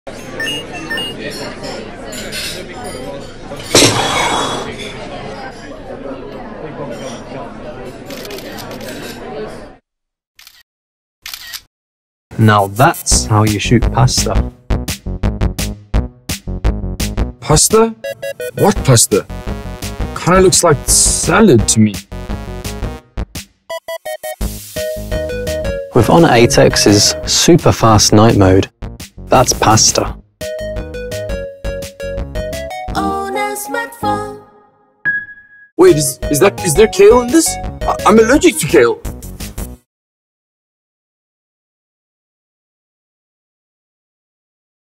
HONOR 8X: Beyond Limits Series- EPISODE1 AI Camera
You might be able to see what you’re eating, but your phone might not! How will this accidental dinner date unfold? Watch the video to find out!

Introducing Our HONOR8X Full Spec
★Dual Texture Reflective Back Glass Design
★ 20MP+2MP Rear Dual Lens AI Camera
★ 6.5’’ Borderless FullView Display with 91% Screen-to-Body Ratio
★ Kirin 710 Processor
★ Enhanced by GPU Turbo